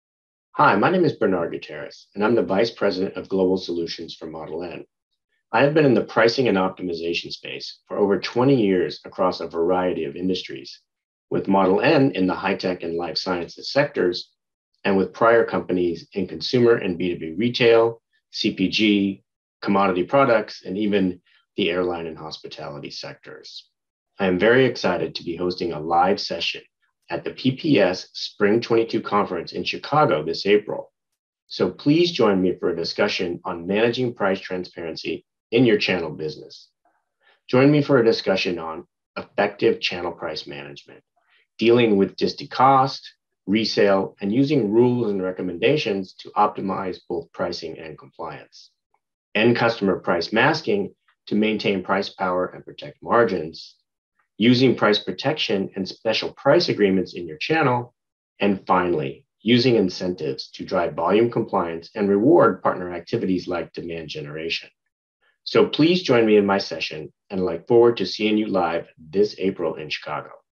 PPSCHI22 Pricing Breakout Speaker Bernard Gutierrez, VP, Solutions, Model N
Bernard Gutierrez, VP, Solutions, Model N will lead a Pricing Breakout Session titled, "Managing Price Transparency Channels" during the Spring Pricing Workshops event PPSCHI22.

High Tech has seen explosive growth in channels from distributors, retailers, and partners, with some reporting 70% of revenues going through the channel. How do you allow the channel pricing freedom without dropping to floor pricing? How do you ensure you are getting the volume promised? How can you best react to market changes? Join us for a discussion on how to effectively manage channel pricing and using incentives to drive volume compliance.

Participants will learn:
- Use price masking to maintain pricing power
Apply incentives to drive volume compliance
- Incentivize channels by using Price Protection and Special Pricing

The Professional Pricing Society is the world's leading professional organization dedicated to pricing training and education. We're the home of Pricing!